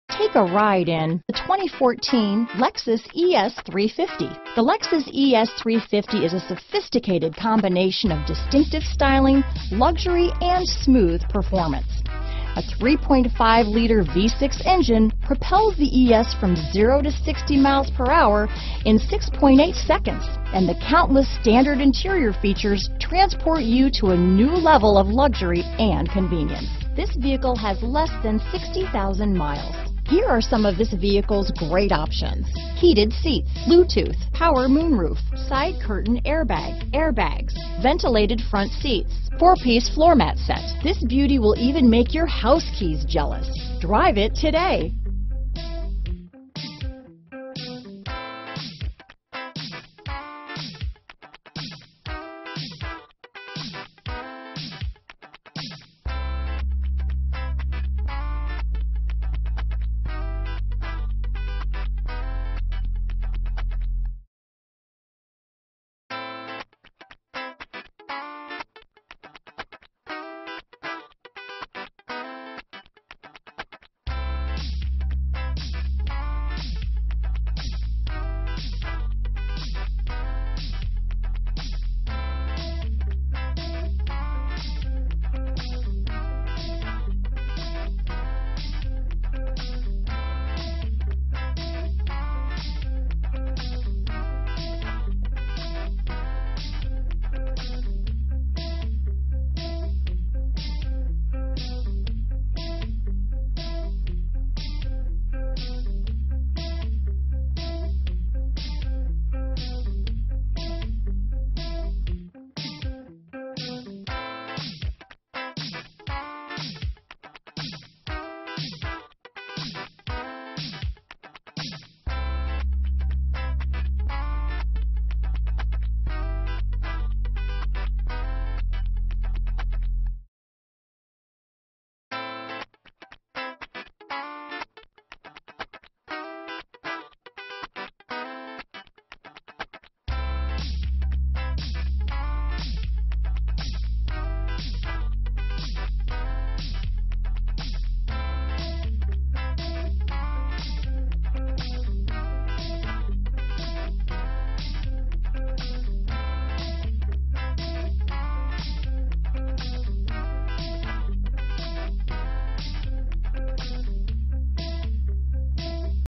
2014 LEXUS ES350 Las Vegas, Henderson, St. George, Cedar City, Summerlin, NV HDT2808D
Obsidian Used 2014 LEXUS ES350 available in Las Vegas, Nevada at Lexus of Henderson. Servicing the Henderson, St. George, Cedar City, Summerlin, NV area.
2014 LEXUS ES350 LUX DSPL AUDIO - Stock#: HDT2808D - VIN#: JTHBK1GG8E2085661

Driver/Front Passenger Dual-Stage Front Airbags,Driver/Front Passenger Knee Airbags,Front & Rear Seat-Mounted Side Airbags,Front & Rear Side-Curtain Airbags,Front Passenger Occupant Classification System,HomeLink Programmable Garage Door Opener,Rear Back-Up Monitor,10-Way Power Adjustable Front Seats,7-Inch Audio Display Screen,8-Speaker Audio System,AM/FM/CD/MP3/WMA Player,Automatic Climate Control System,Auxiliary & USB/iPod Audio Input Plugs,Bluetooth Connectivity,Drive Mode Select,Driver Seat/Mirror/Steering Memory,Electrochromic Auto-Dimming Interior Mirror,Genuine Wood Trim,HD & SDARS Radio Capability,Heavy Duty Rear Window Defogger w/Timer,Nuluxe Seats,Power Tilt/Telescopic Steering Wheel,Satellite Radio Capability,Heated Seat(s),Air Conditioned Seats,Exterior Mirror Puddle Lamps,Integrated Foglamps,Power Heated Exterior Mirrors w/Signals,Power Moonroof,SmartAccess w/Push-Button Start/Stop,Washer Linked Intermittent Wipers w/Timer,Electronic Brake Distribution & Brake Assist,Power 4-Wheel Disc Anti-Lock Brakes,Traction Control & Vehicle Stability Control